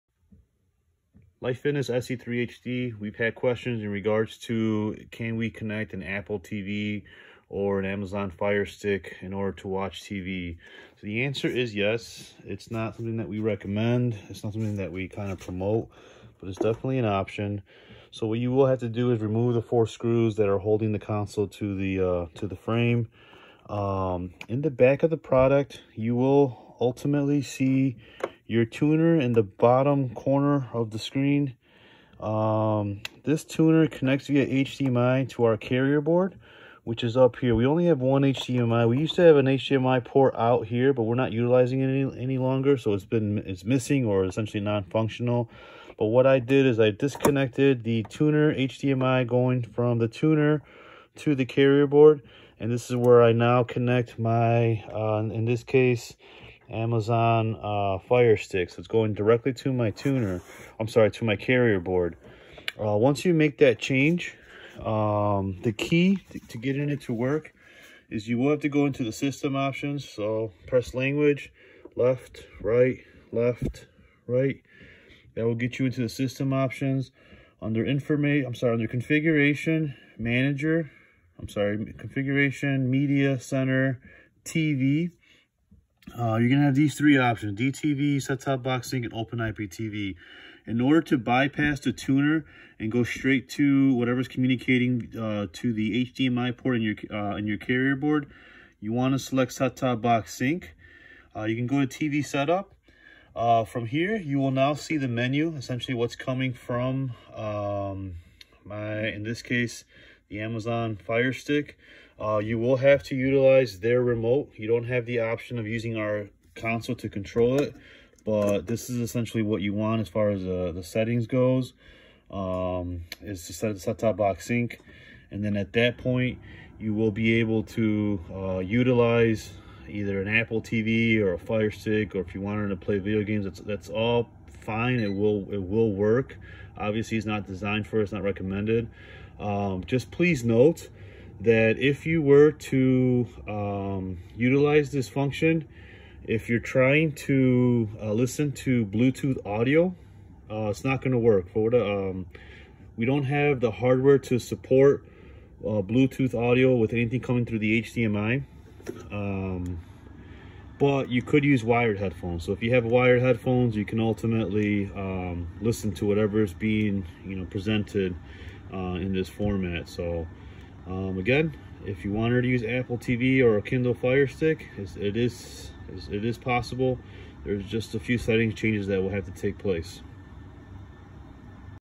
How to connect Apple TV or Firestick to Life Fitness SE3HD
This video shows how to connect Apple TV or Firestick to Life Fitness SE3HD console. TV settings will need to be changed to STB SYNC. The use of their remote is needed and audio will only work with wired headphones